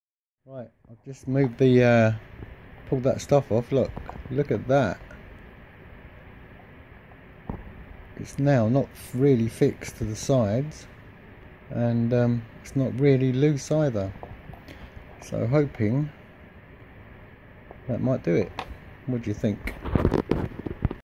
Anti jello quick fix.
Pulled the camera away from the ply mount it was glued to with UHU POR which is like a rubbery glue. The unit now hangs loosely but not too loose, it's not touching the frame either, the glue is suspending it, will see if it has fixed the jello issue next time out.